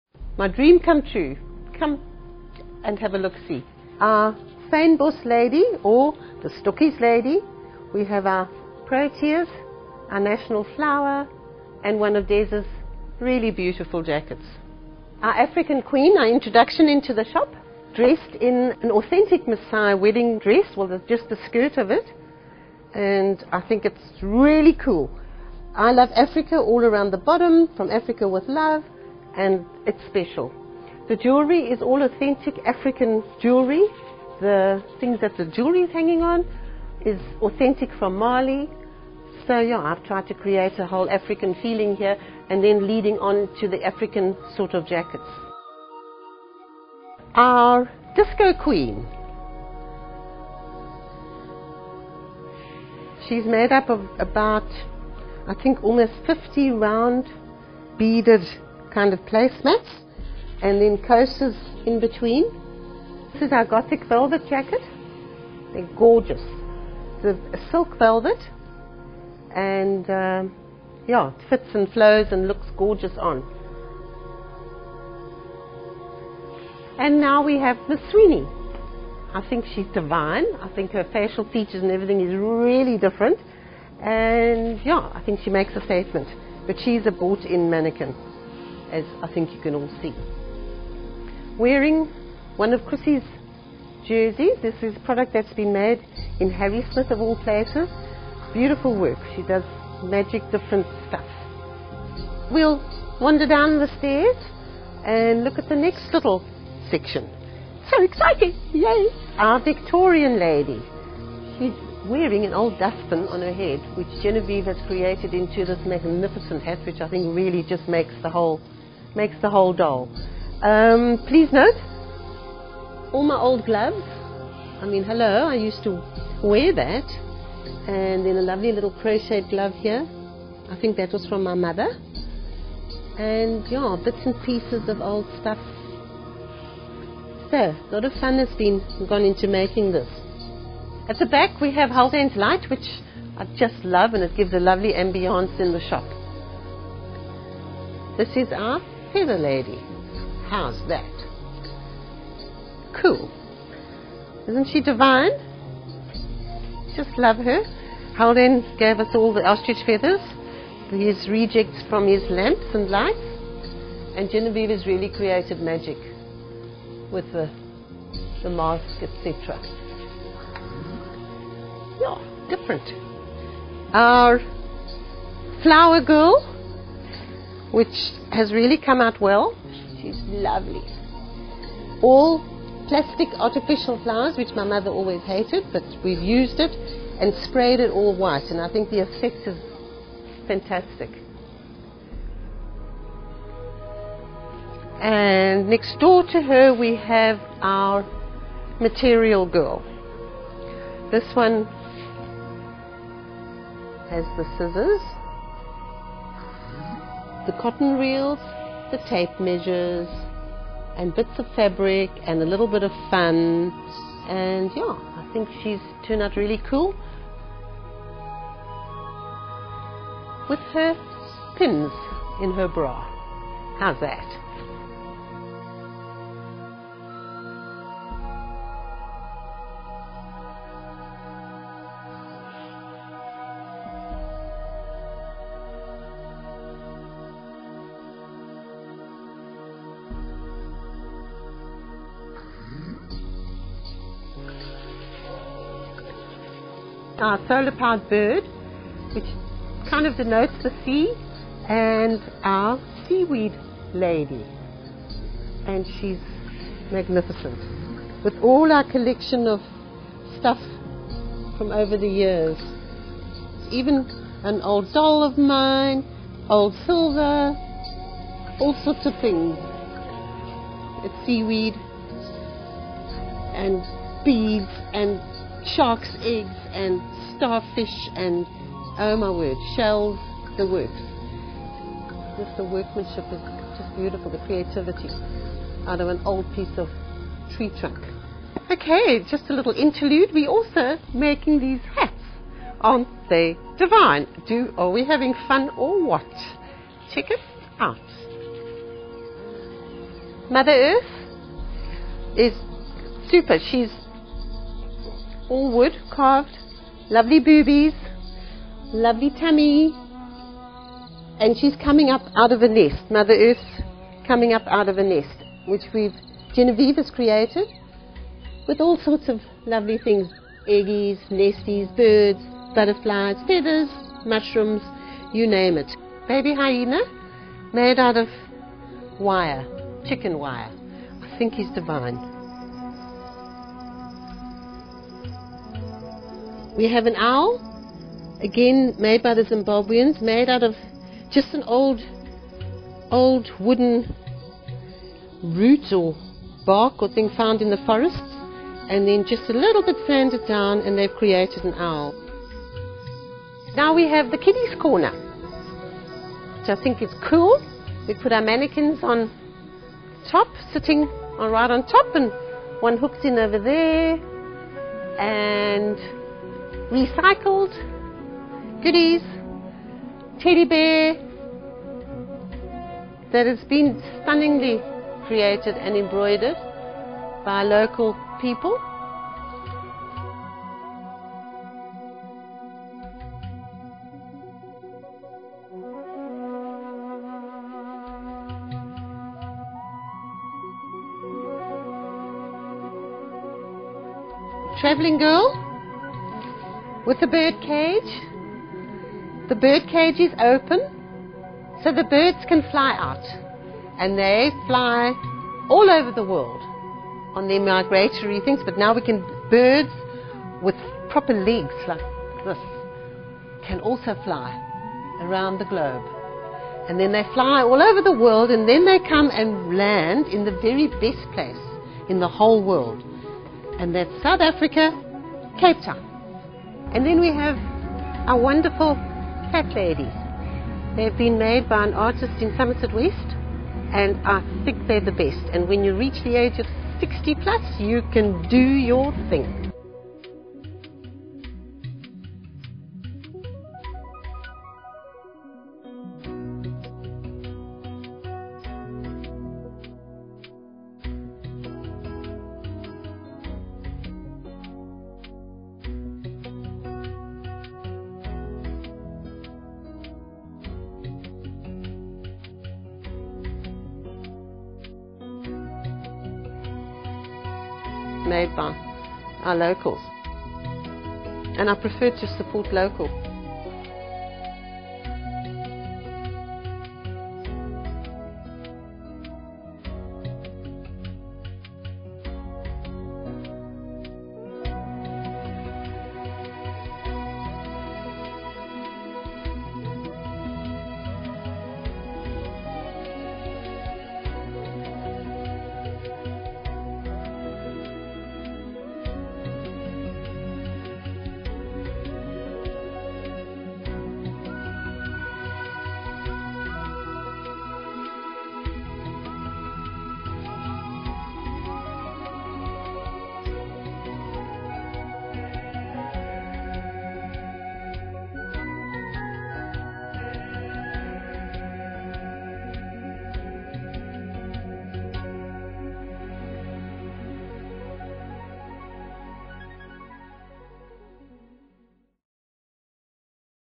Solveig's Shop
A short video showing the mannequins and other props in Solveig's new Gallery of Wearables shop at 4 Alfred Mall, V&A Waterfront in Cape Town.